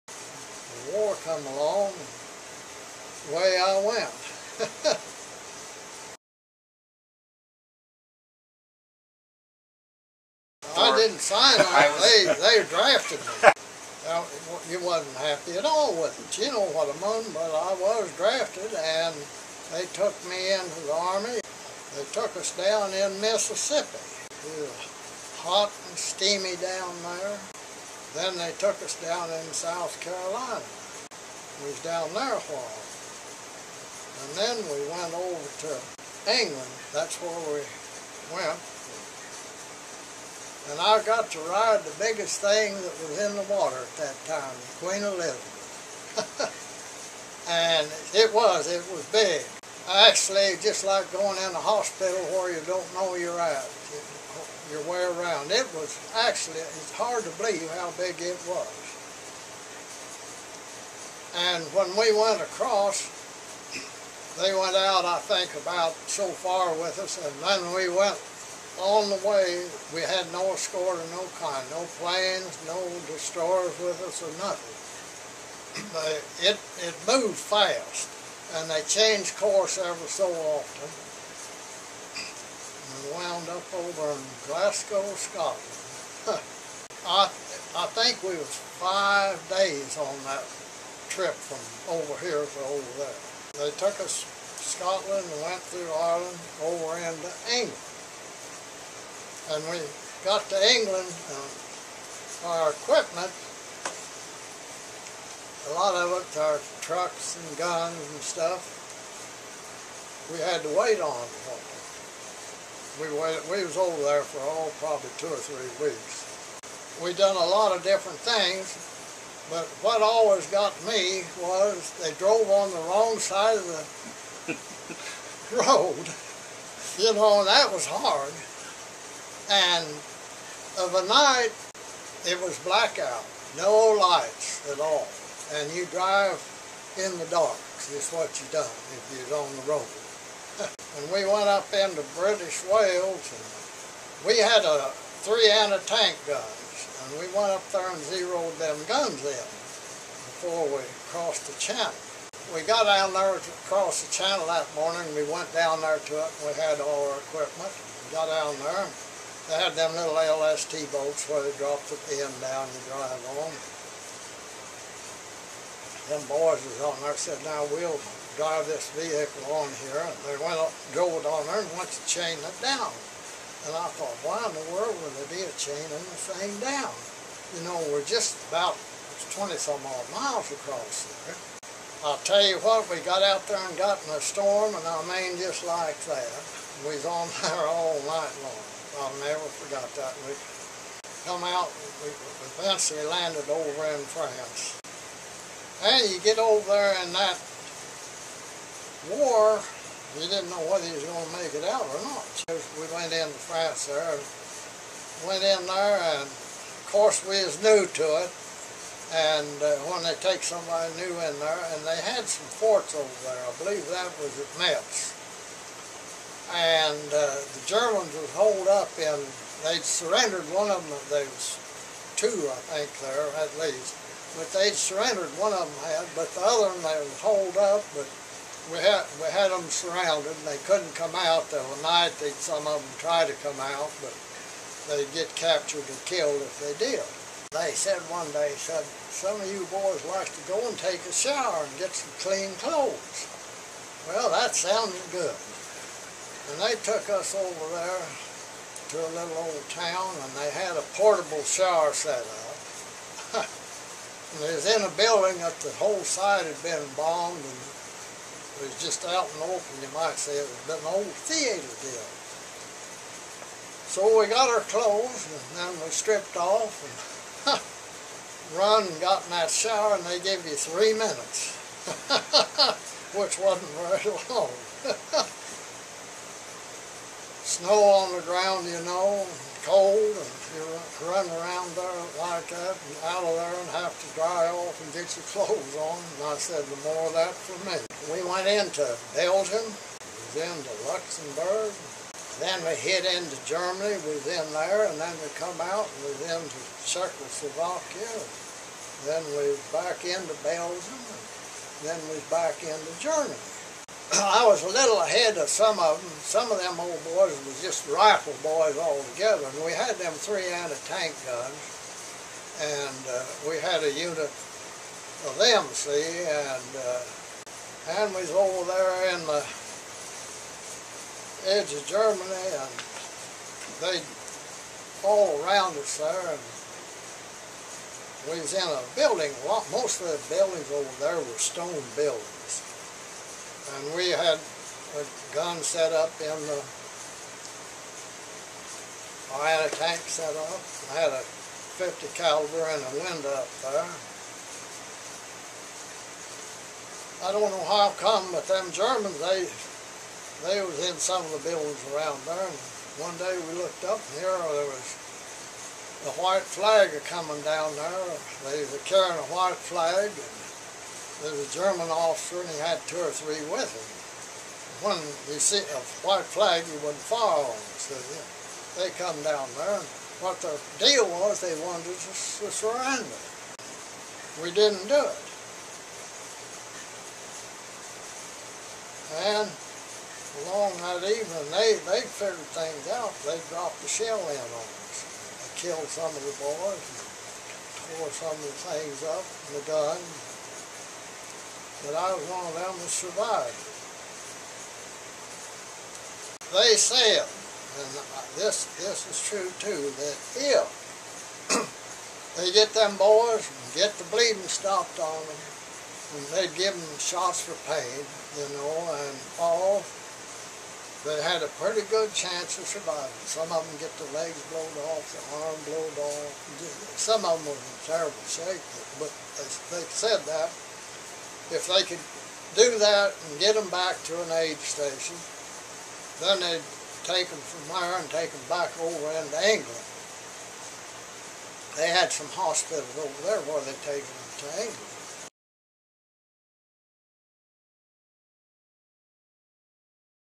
Fred Powell — Off to war
World War II veteran Fred Powell recounts memories of going to war.